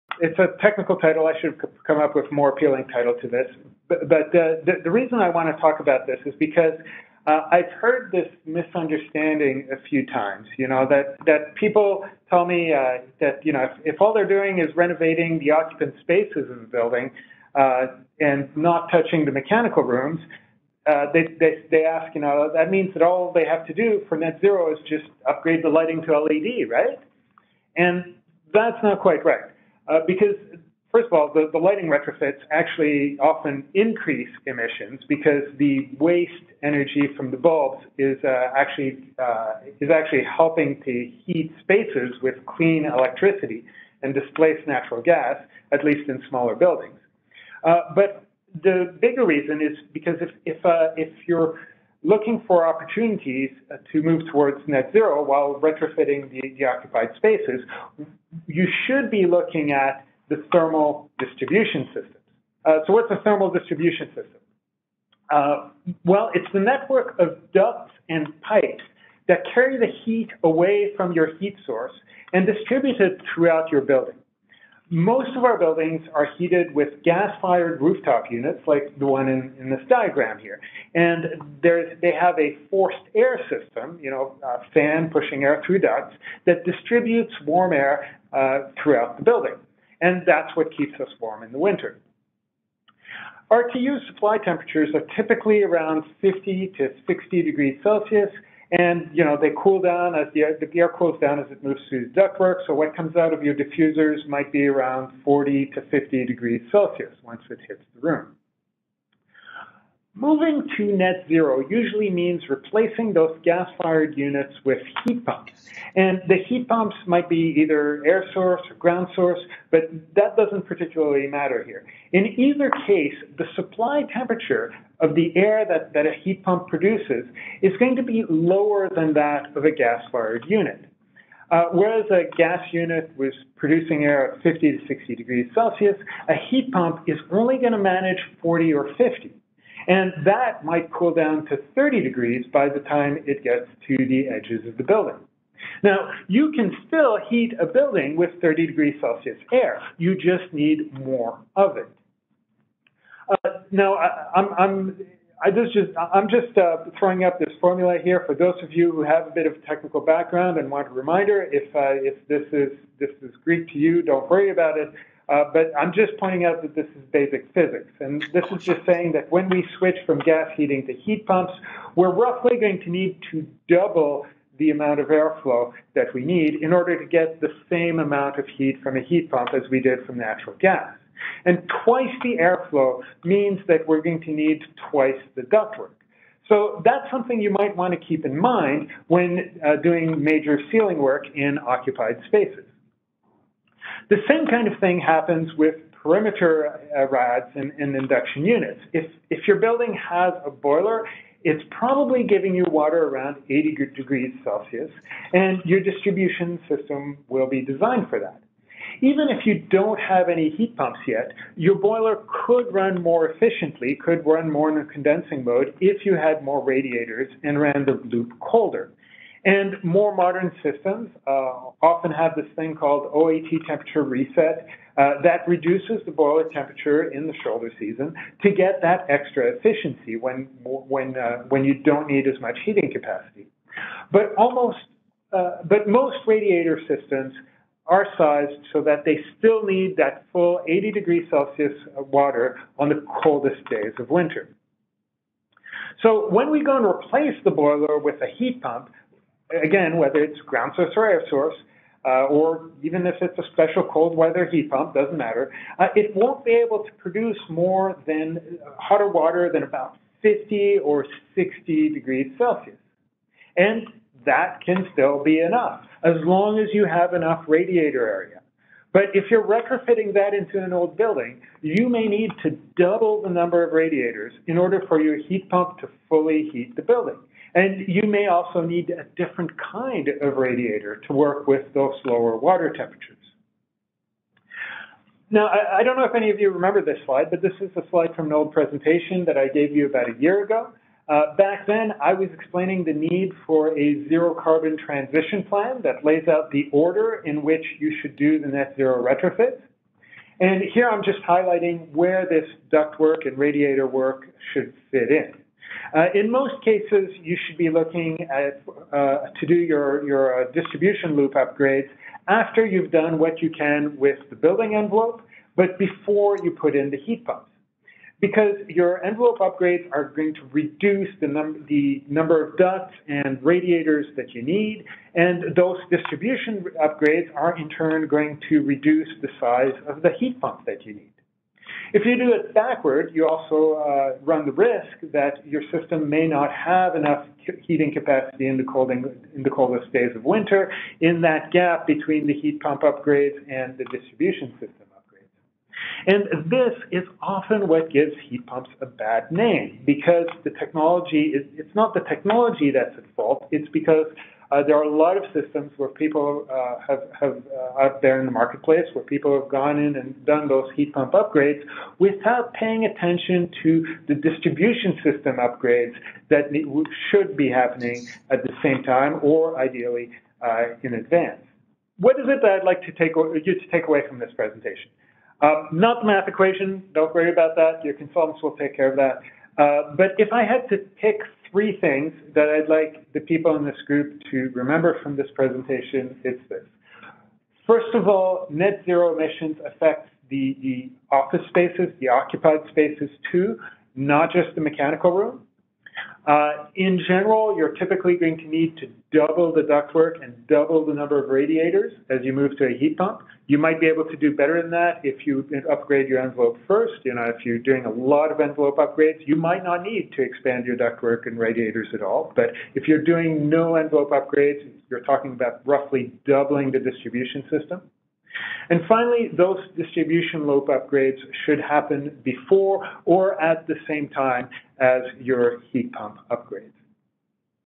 Distribution Loop Temperatures
Before installing a heat pump, consider whether you may need upgrades to ductwork and radiators. Heat pumps produce lower temperatures than natural gas systems do. That doesn't limit the amount of heat they can provide, but it does mean you have to increase ductwork or radiator area to compensate.